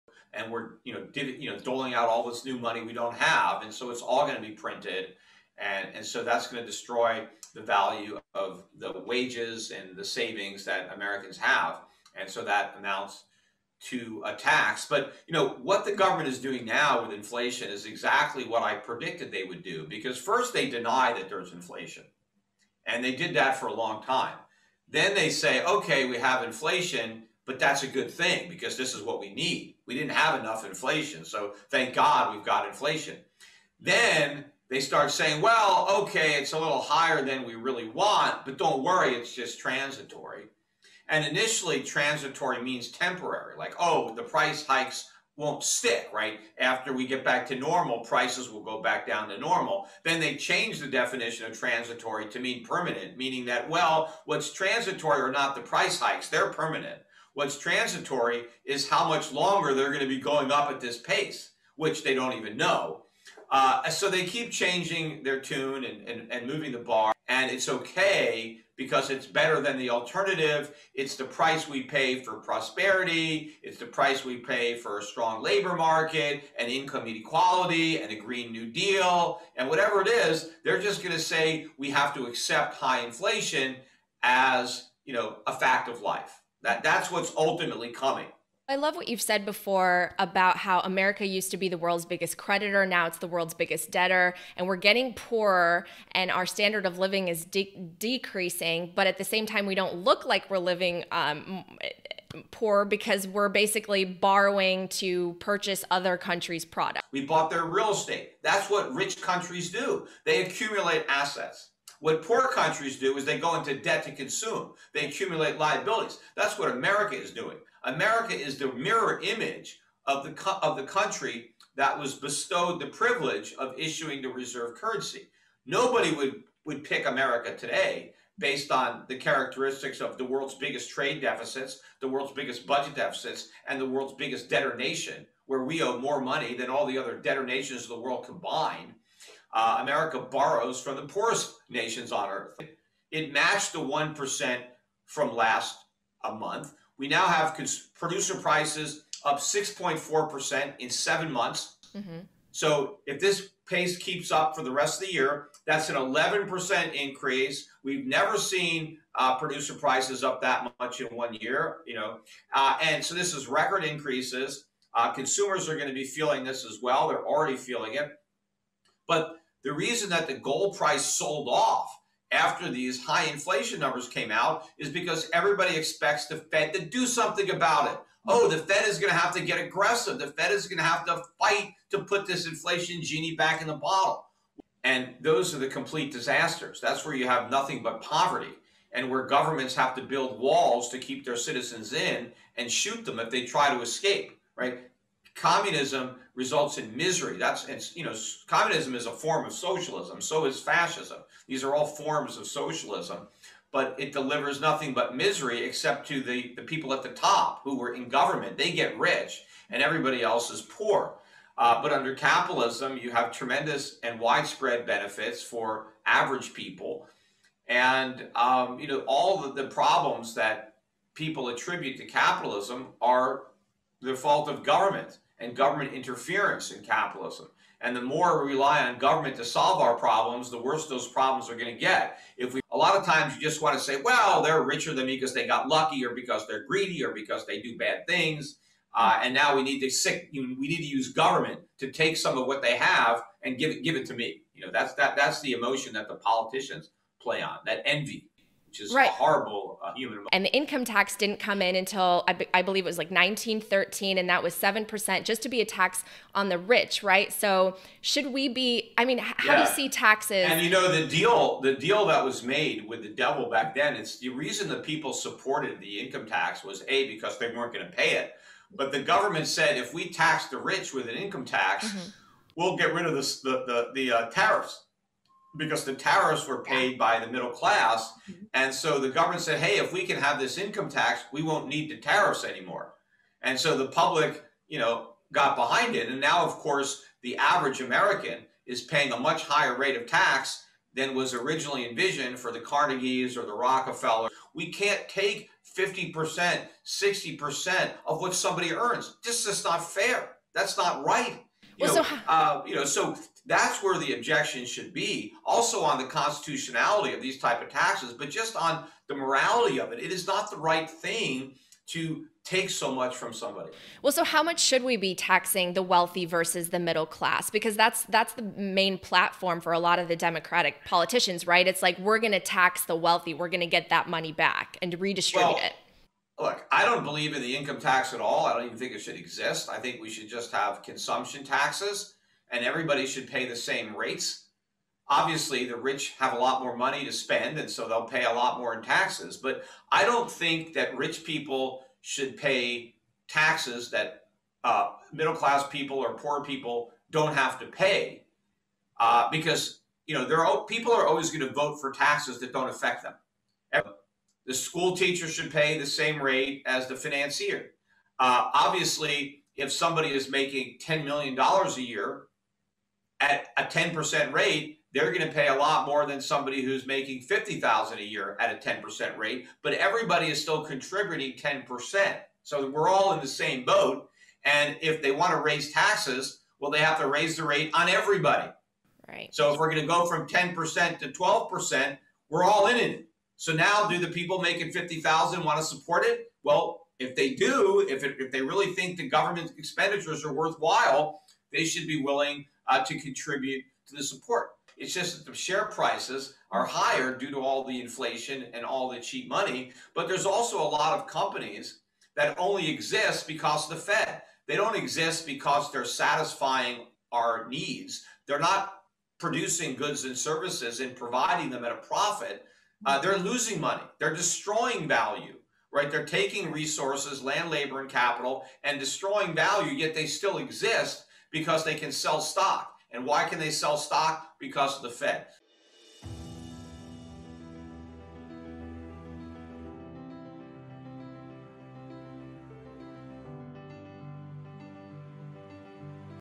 Peter Schiff- Is This The End Of Bitcoin Boom?
Peter Schiff, the famous economist and financial expert, predicts a Bitcoin crash in 2018. He argues that the U.S. dollar will be worth more than Bitcoin by this time next year!